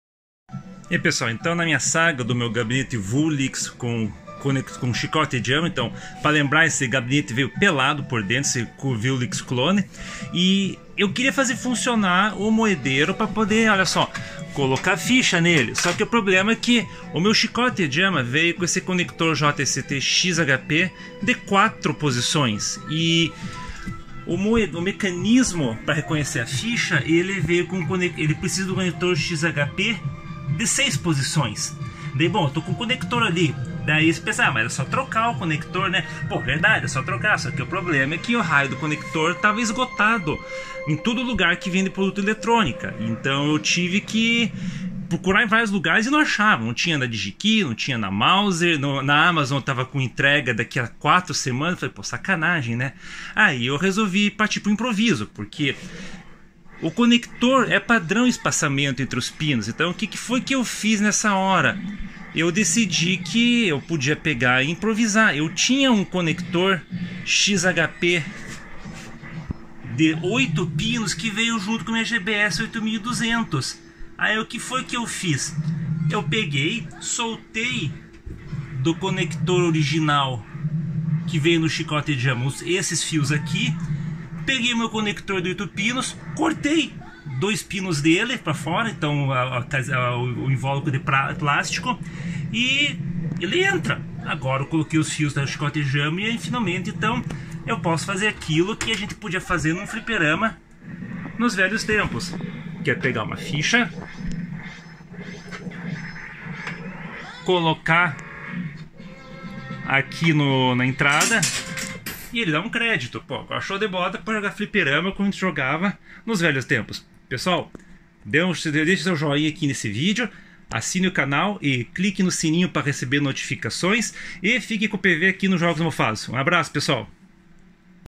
Se Virando Pra Usar o Moedeiro - VLog de como Resolvi um Conector Esgotado para usar o Fliperama
Quando comprei a minha Vewlix, ela veio pelada por dentro, sem nenhum cabo ligando nada. Aí coloquei um chicote JAMMA pra usar placas clássicas na máquina, mas ainda não tinha usado o mecanismo pra aceitar fichas. Isso porque o conector do moedeiro no chicote JAMMA era de 4 posições, mas o conector que eu precisava era de 6 pinos.

Como o conector JST XHP de 6 posições está esgotado nas lojas que eu costumo comprar, tive que me virar como dava e achei uma solução satisfatória que vaiser permanente. Agora, é só meter ficha!